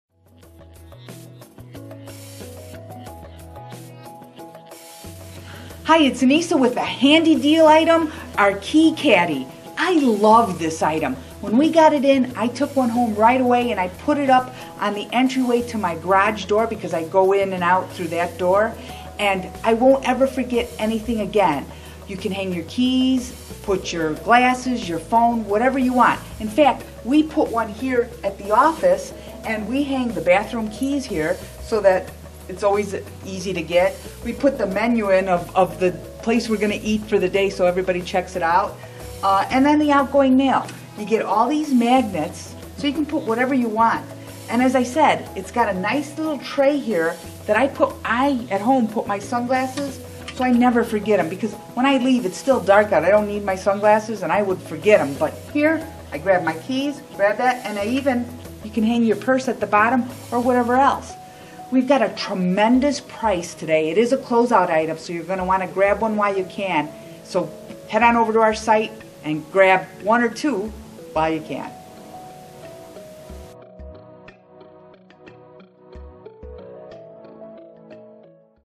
Hanging Key Caddy
Everything is in modern-looking brushed stainless. Another nice feature of this wall organizer are the sturdy mounting brackets on the back. You hang this on a couple of wall anchors and it is not coming off.